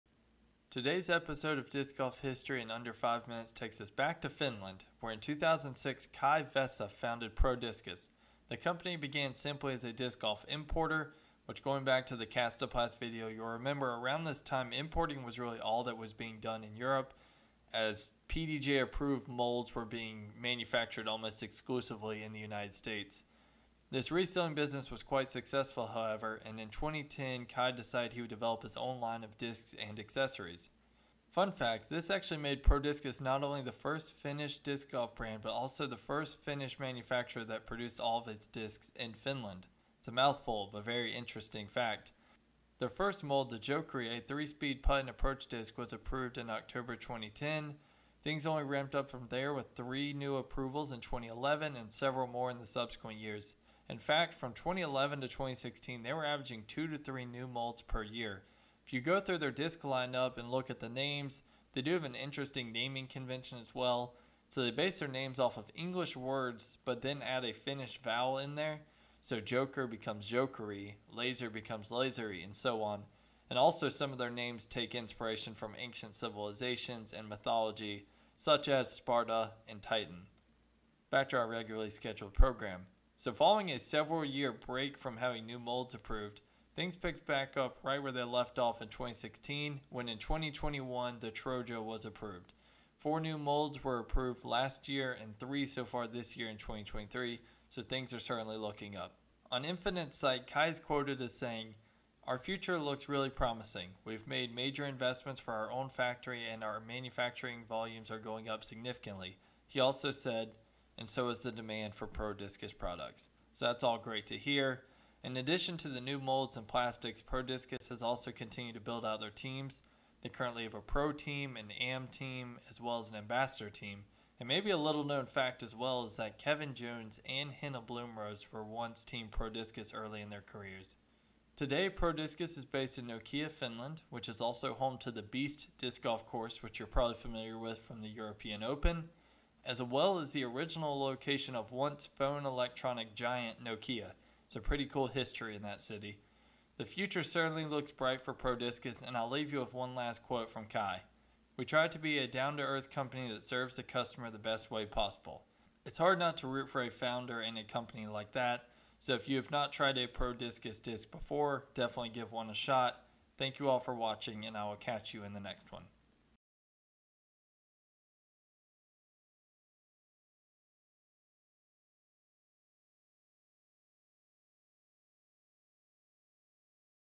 Prodiscus - Disc Golf History in under 5 minutes!
The fourth installment in our new series "Disc Golf History in under 5 minutes!" where we cover the history of brands, discs, tournaments, etc in under 5 minutes.. bite-sized content if you will..

Next up is Prodiscus from Finland!

From the company: "Prodiscus is the first Finnish disc golf brand and manufacturer that produces all it’s discs in Finland. Prodiscus started in 2006 as a disc golf product importer. Prodiscus’ founder Kai Vesa decided that he will create and develop own line-up of discs and accessories for the disc golf community. Since starting, we have provided high quality discs, bags and equipment to the players. We’re known for our high-quality products worldwide."